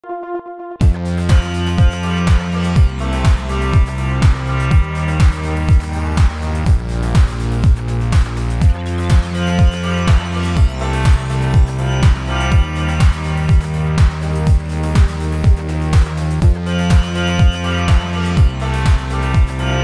BR50
BR50 058 z tendrem kabinowym ze składem osobowym kolei włoskich FS